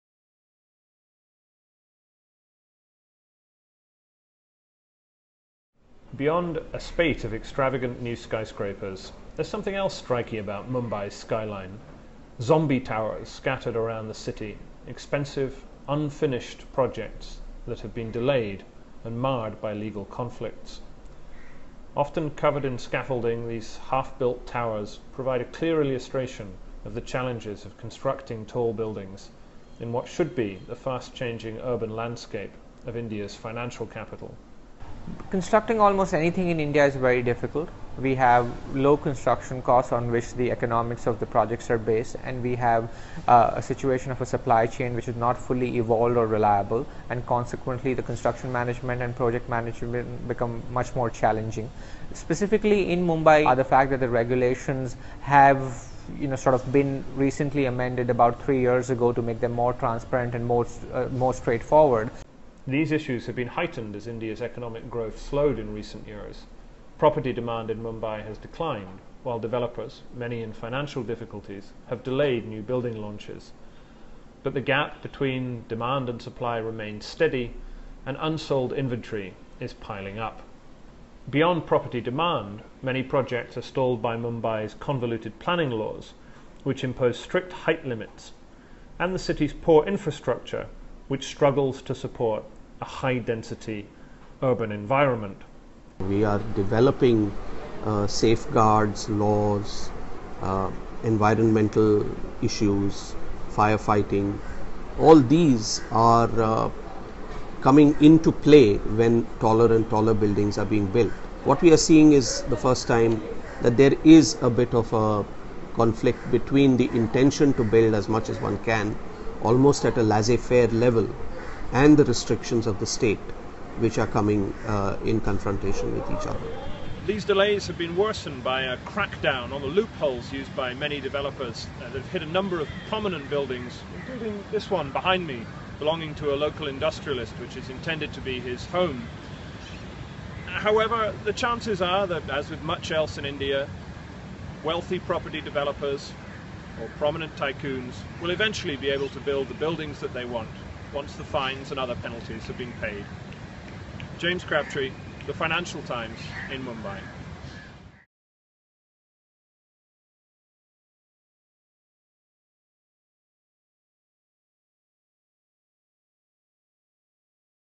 Mumbai's zombie towers | FT Life
The skyline of India's financial capital should be the home of some impressive new buildings. Instead it has a series of unfinished towers draped in scaffolding as developers delay new building launches. The FT's James Crabtree examines why.